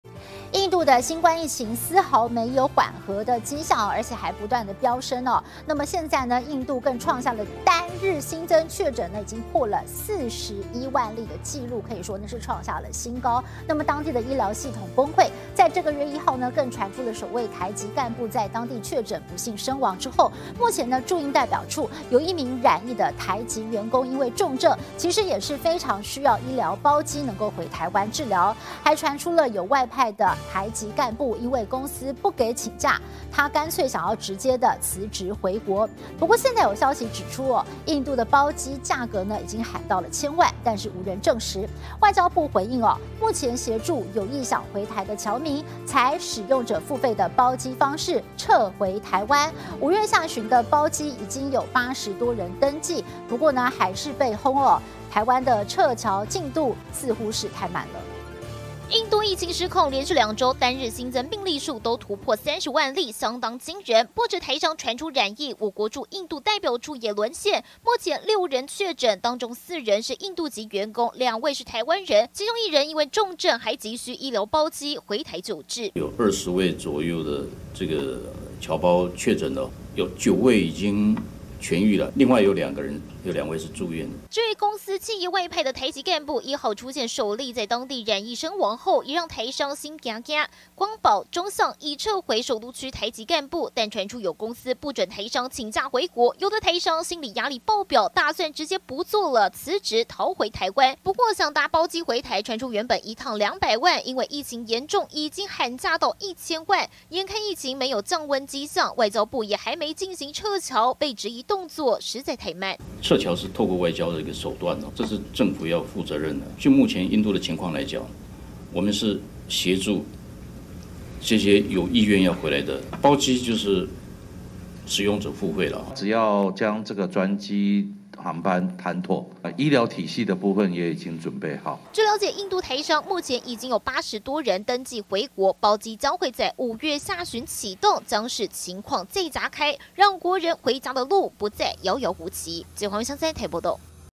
疫情狂燒! 光寶.中橡撤回首都區台籍幹部
印度新冠疫情沒有降溫跡象，過去兩周每日新增病例都超過30萬，當地醫療系統崩潰，這個月1號傳出首位台籍幹部在當地確診身亡後，目前駐印代表處一名染疫的台籍員工因重症，也急需醫療包機回台治療。還傳出有外派的台籍幹部，因為公司不給請假，想乾脆辭職回國。不過有消息指出，印度包機的價錢已經喊到千萬，但無人證實。外交部的回應是，目前協助有意想回台的僑民，採使用者付費的包機模式，撤回台灣，五月下旬的包機已經有80多人登記。疫情指揮中心則表示，航班談妥，國內醫療體系也會準備好進行協助。